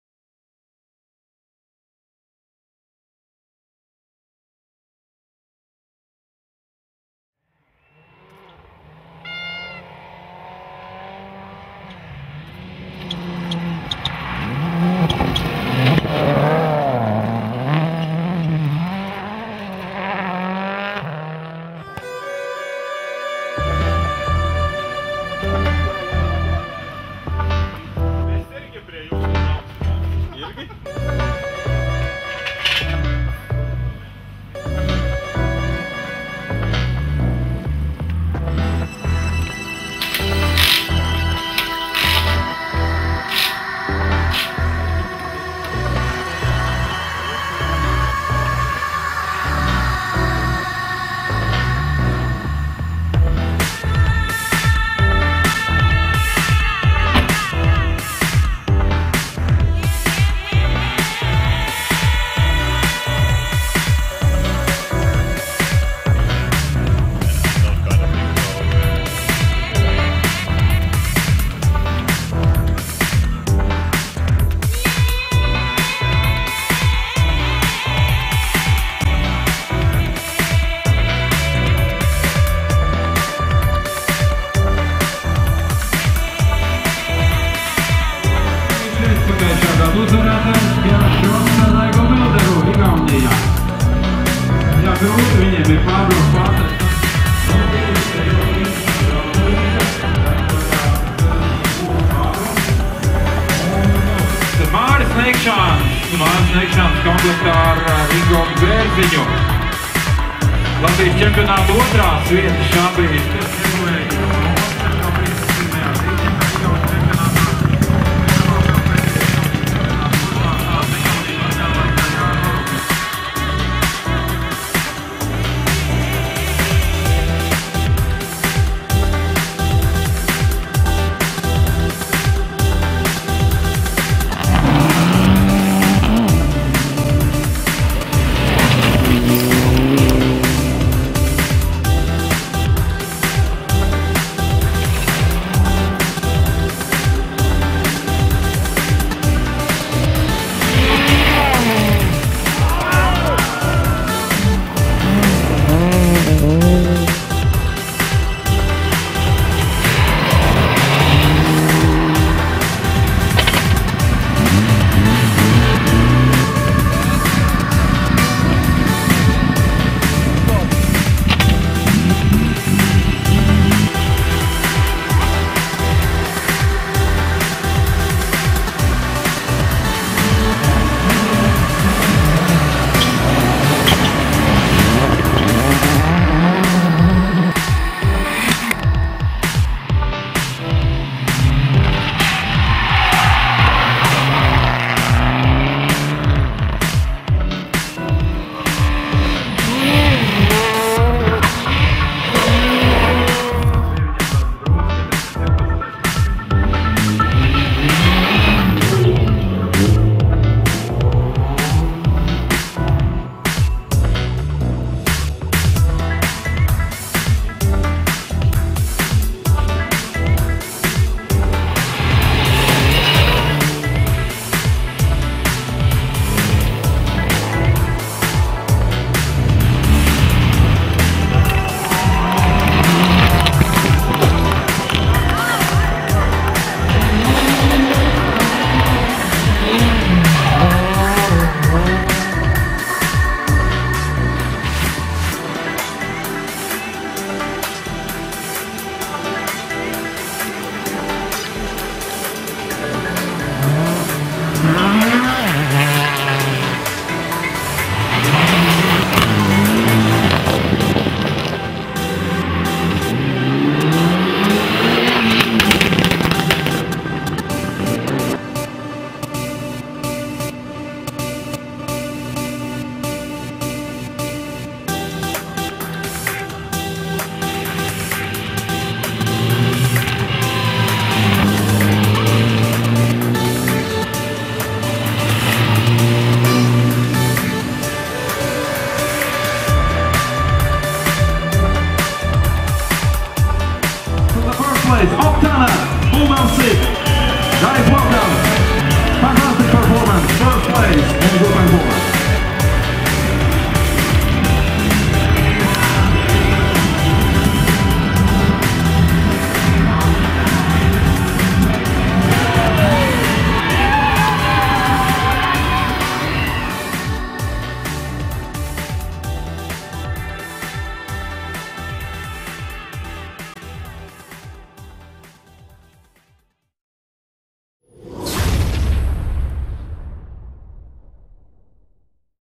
Rally KURZEME 2011
Service, Opening, Spectators, Rally and Awards! Allf from rally Kurzeme 2011!